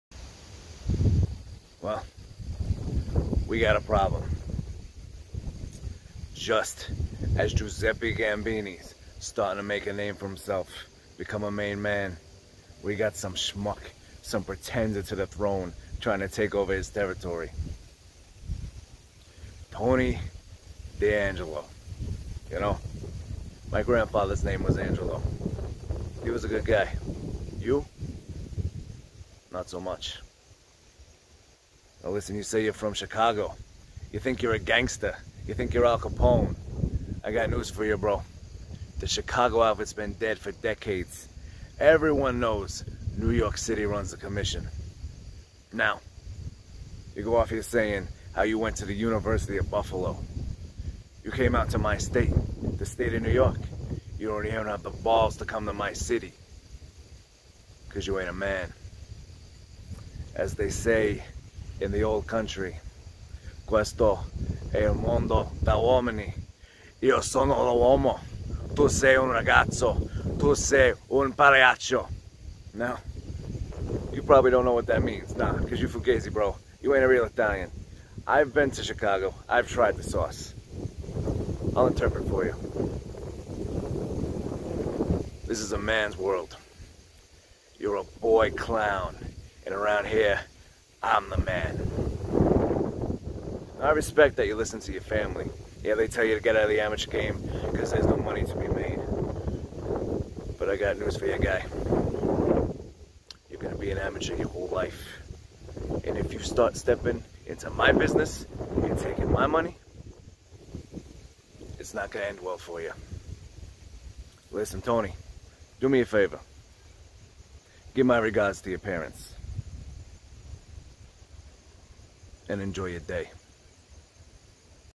Sending a warning to Tony D'Angelo
This fugazi schmuck in nxt is moving in on my turf.